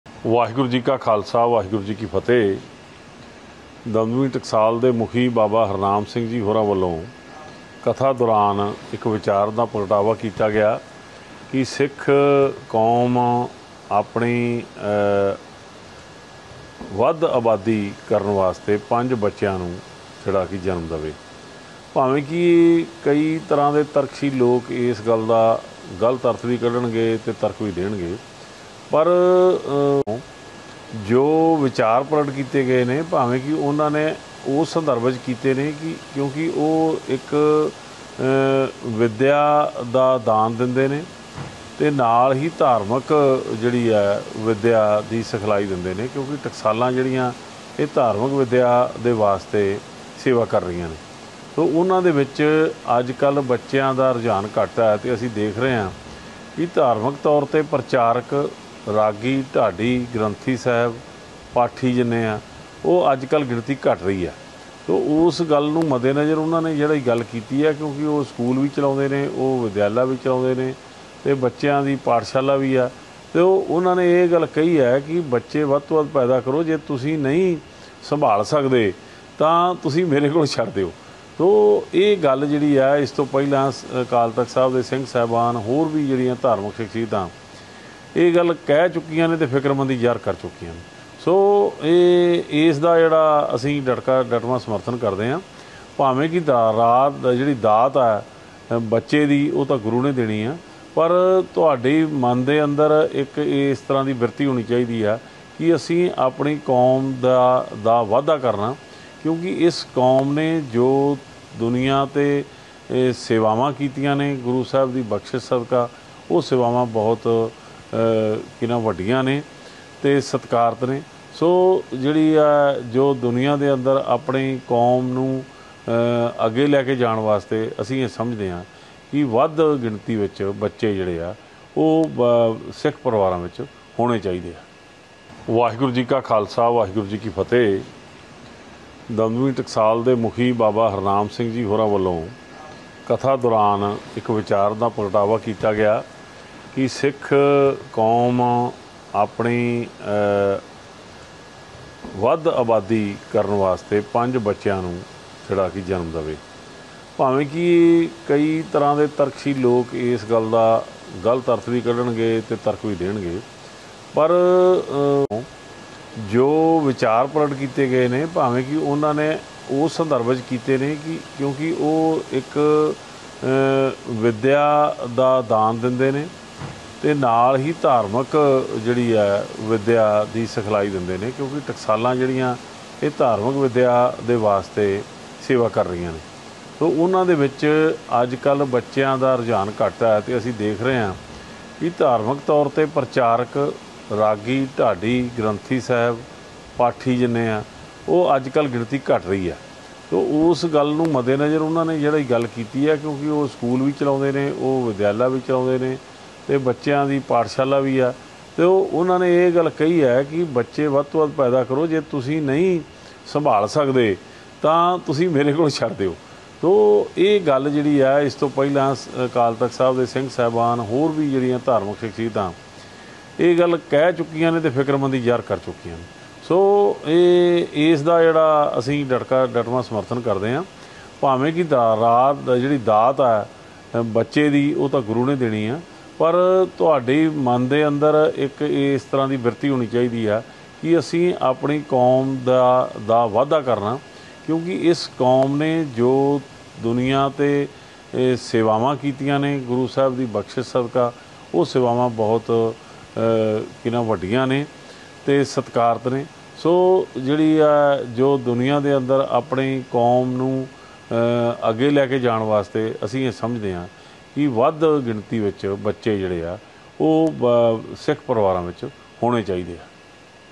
Baba Harnam Singh ਦੇ ਬਿਆਨ ਦੀ SGPC ਮੈਂਬਰ ਵੱਲੋਂ ਹਮਾਇਤ | Gurcharan Grewal | N18V
Baba Harnam Singh ਦੇ  '5-5 ਬੱਚੇ ਪੈਦਾ ਕਰਨ ਸਿੱਖ'  ਵਾਲੇ ਬਿਆਨ ਦੀ SGPC ਮੈਂਬਰ ਵੱਲੋਂ ਹਮਾਇਤ